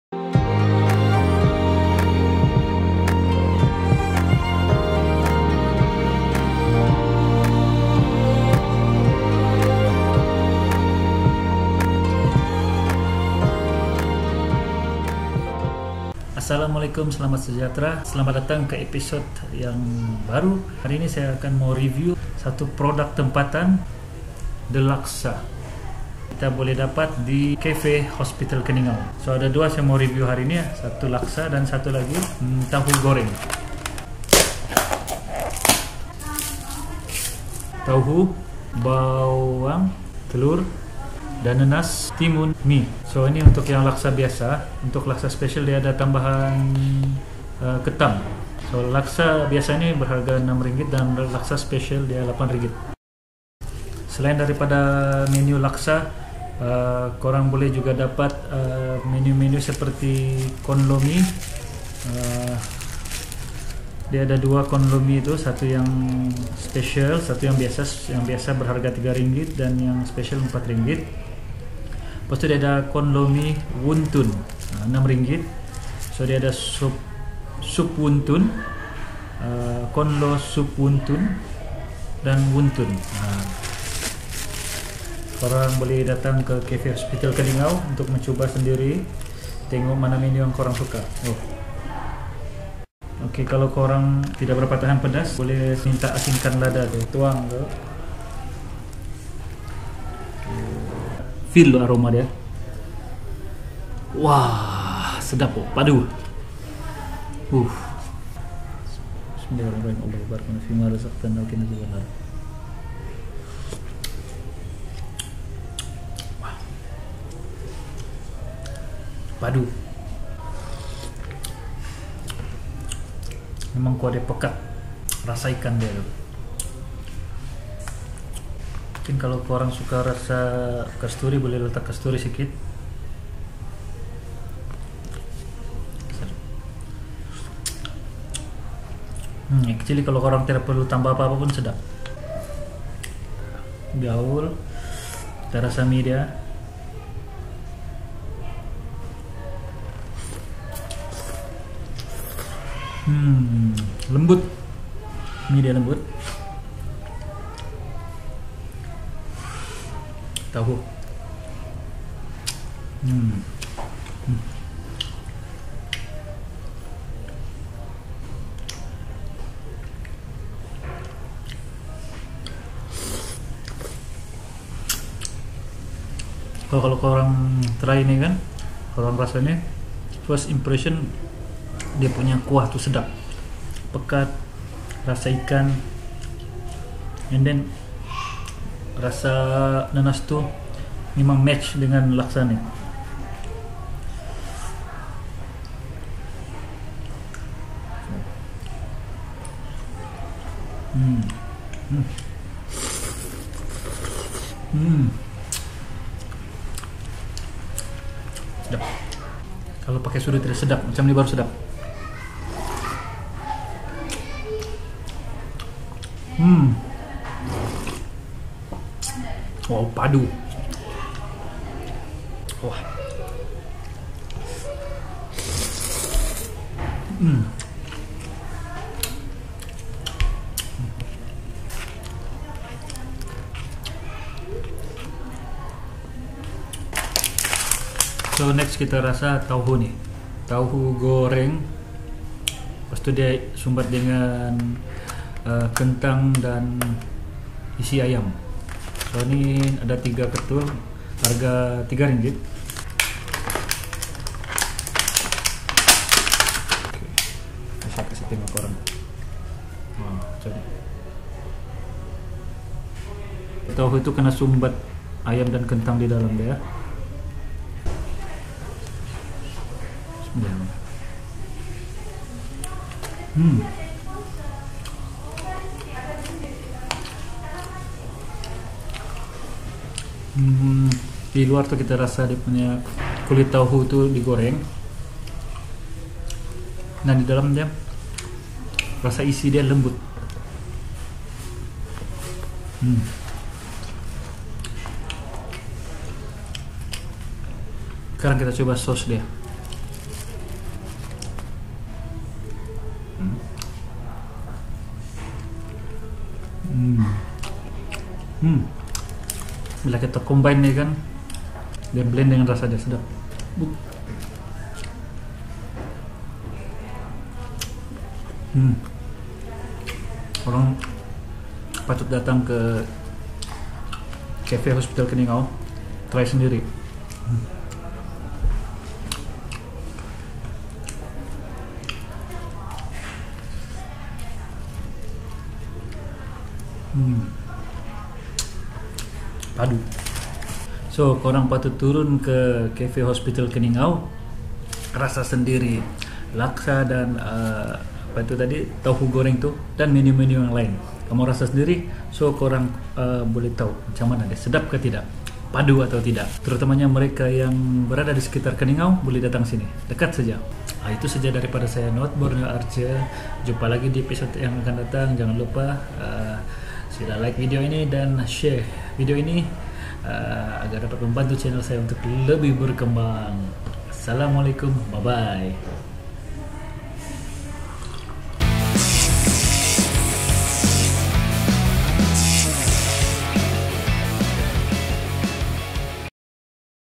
The Laksa Hospital Keningau Cafe
The Laksa merupakan brand satu produk tempatan. Anda boleh menikmati menu ini di Hospital Keningau Cafe pada Isnin sehingga Jumaat sahaja. Anda juga boleh mencuba menu lain seperti Kon Loh Mee dan sup wuntun.